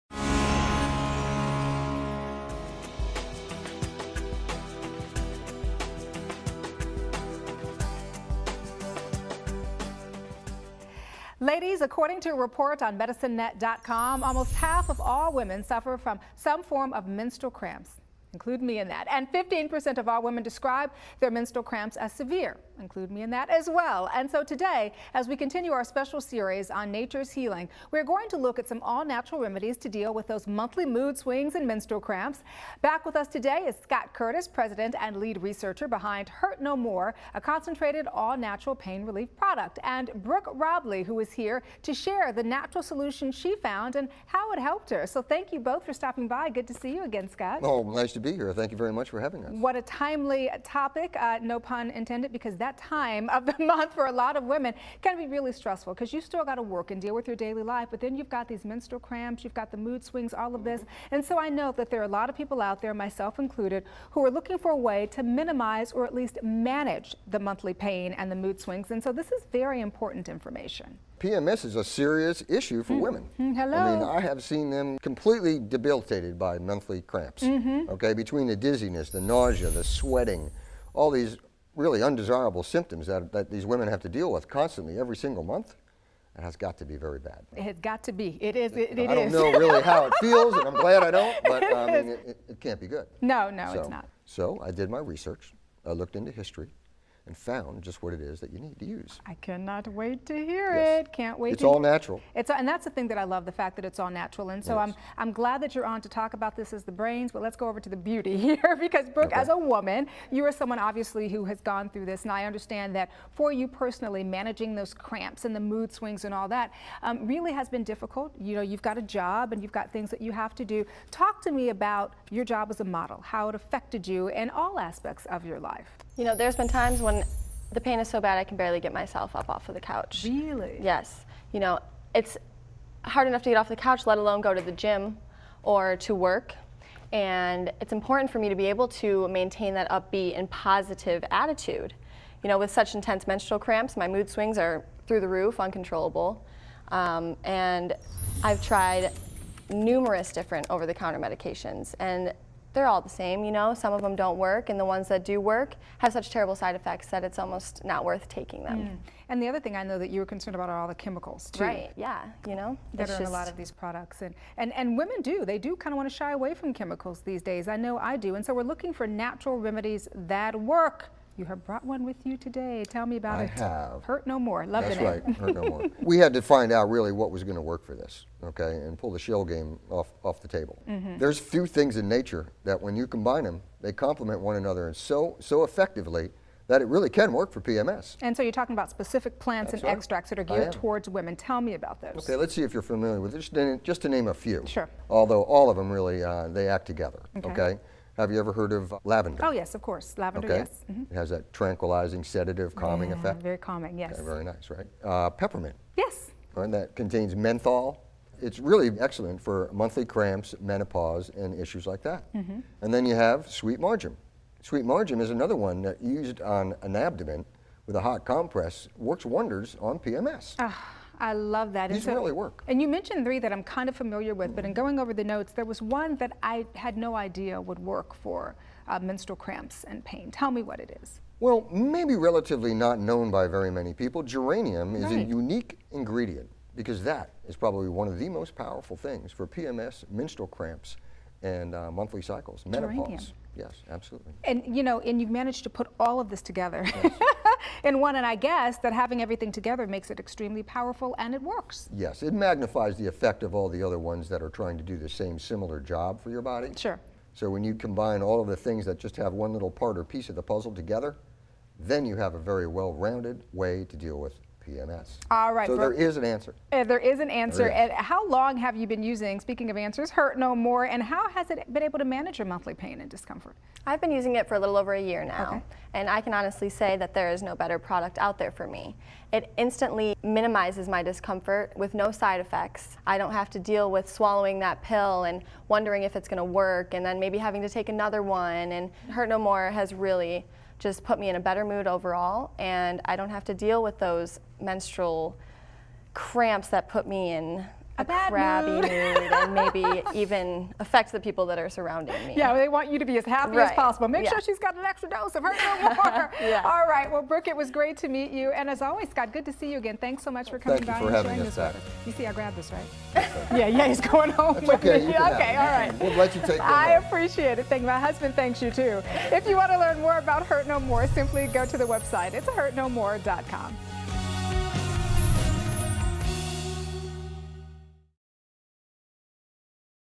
Nature's Healing: Menstrual Cramps and Mood Swings
Find out about an all-natural solution to help ease those monthly cramps and mood swings, in the 2nd part of our special series "Nature's Healing."